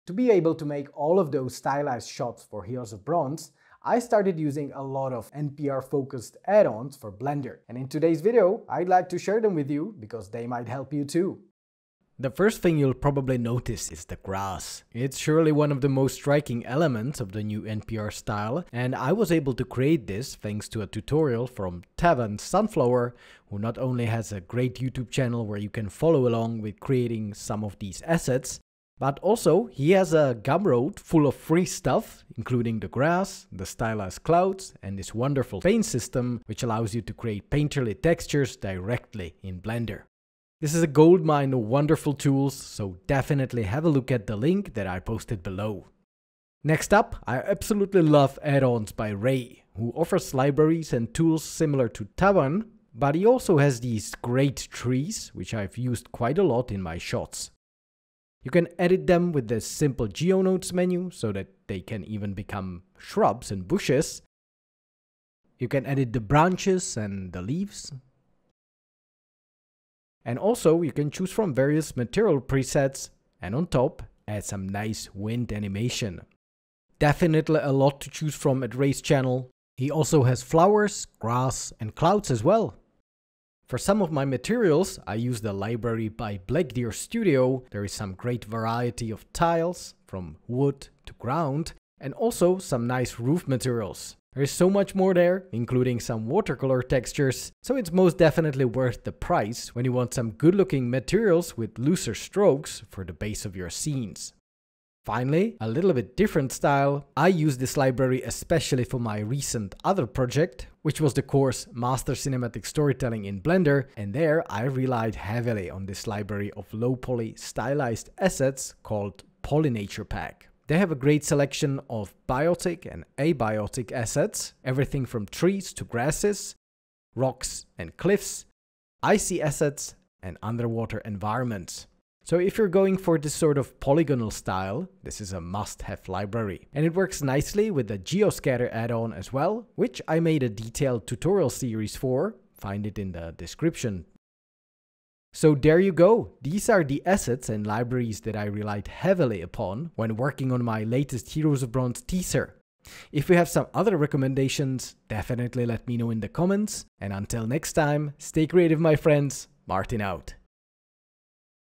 Must-have Stylized Assets for Blender
In this video, I break down the stylized Blender assets and addons I used for my latest Heroes of Bronze teaser in the new NPR workflow.

From Tawan's free Gumroad tools (grass, clouds, painterly texture system), to Rei's stylized trees with GeoNodes controls, BlackDeer Studio’s material library, and the Poly Nature Pack for low-poly environments. These libraries helped me quickly build painterly natural scenes without sacrificing style—links are below if you’d like to explore them too.

Timestamps:
00:00 - Intro
00:11 - Tawan Sunflower
00:46 - Rei's Store
01:23 - Stylized Materials
01:45 - Poly Nature Pack